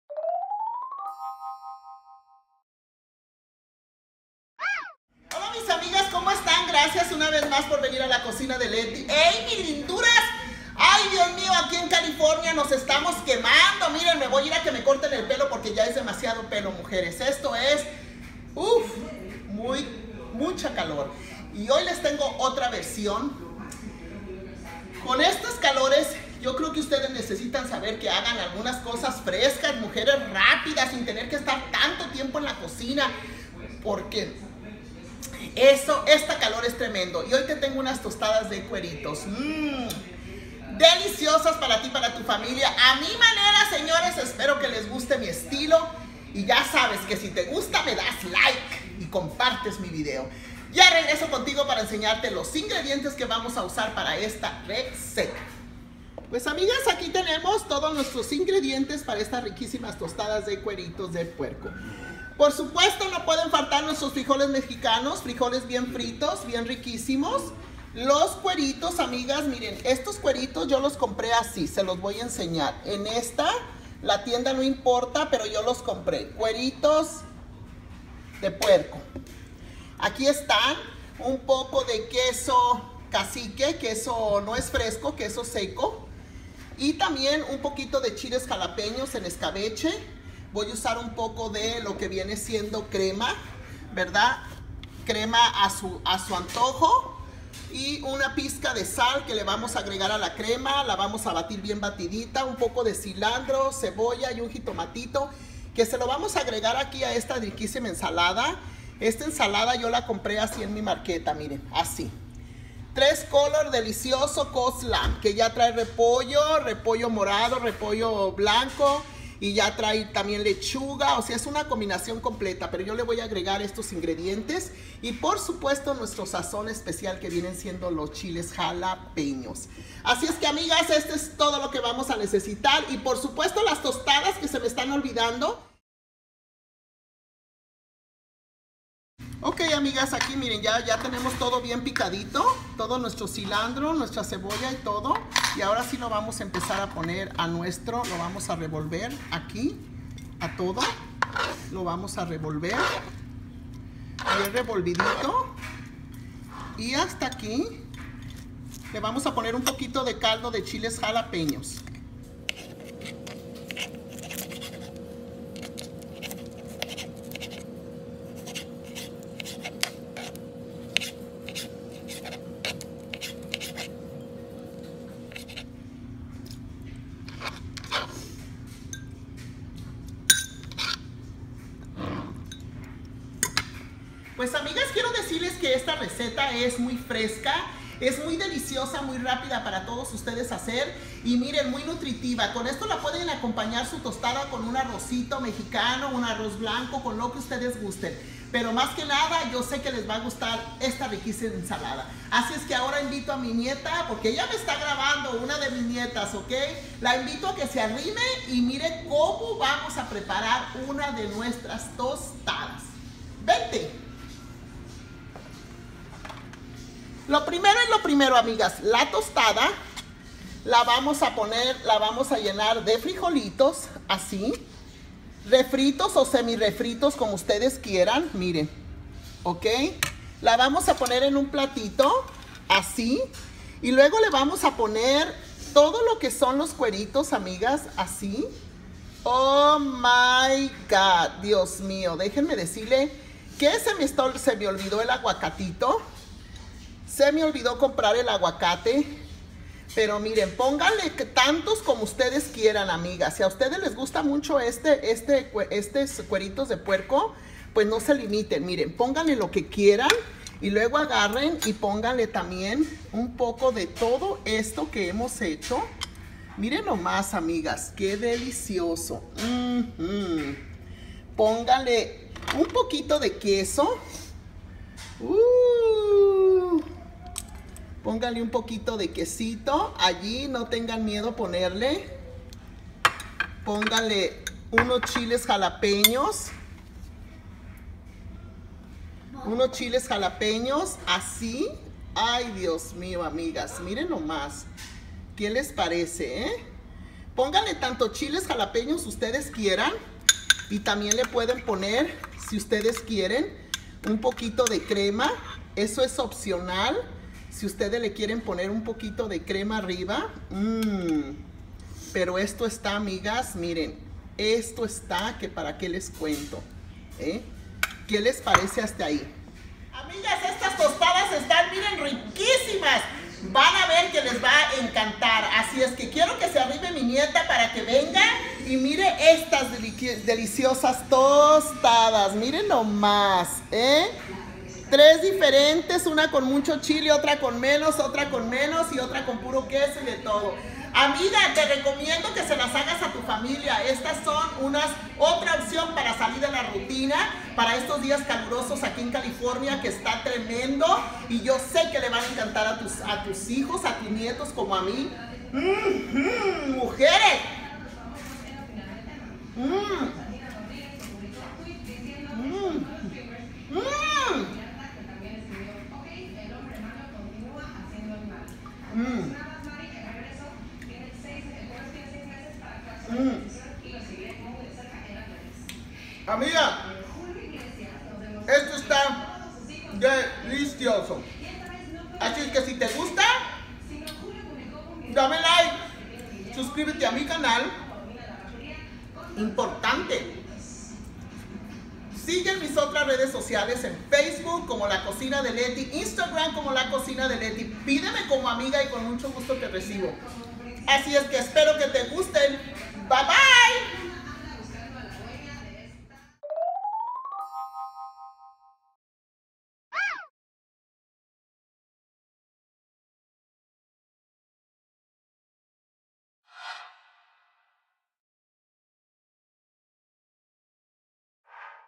Receta de Tostadas de Cueritos para La Calor
❤A QUI LES PRESENTO MI NUEVA Y RIQUÍSIMA RECETA❤

' TOSTADAS DE CUERITOS PARA LA CALOR ' #287

SUBSCRIBANSE AQUI

COMPRAR MANDILES AQUI!!

HABLEMOS EN FACEBOOK =]

❤DIOS LOS BENDIGA Y NO ME ENVIDIEN A PONERSE A COCINAR!!❤